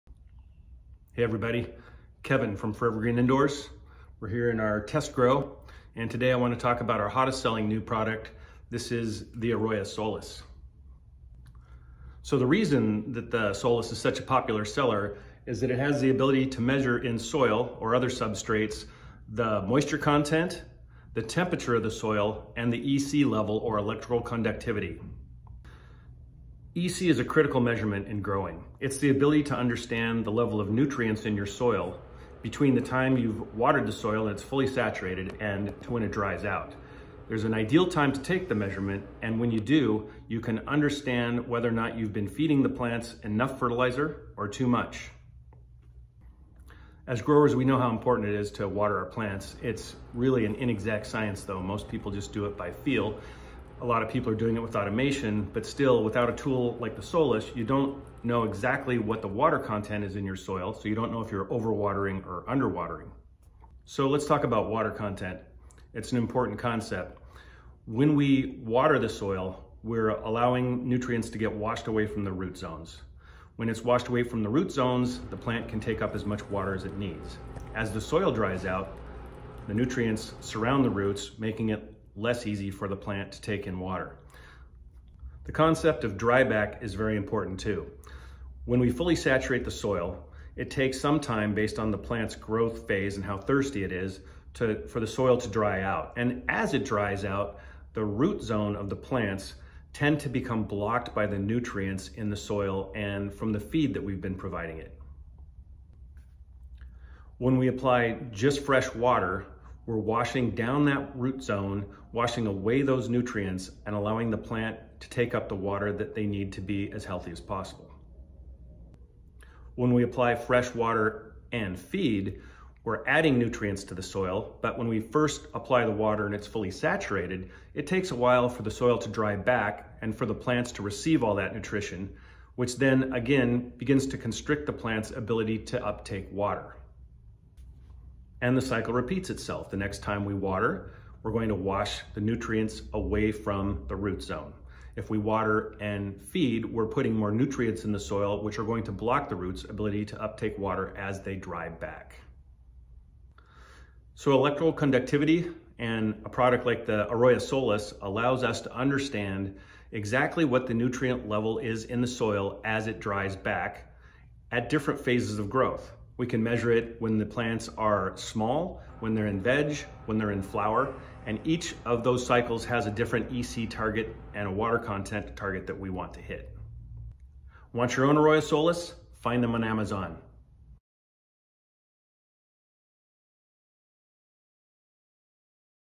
AROYA SOLUS with Teros 12 sensor. Learn how the Solus works.
AROYA SOLUS Bluetooth moisture content meter and EC sensor overview by Forever Green Indoors, an official SOLUS distributor.

Find the SOLUS on Amazon, Ebay, Walmart, Google Shopping and at Forever Green Indoors.

On Sale Today. Save $25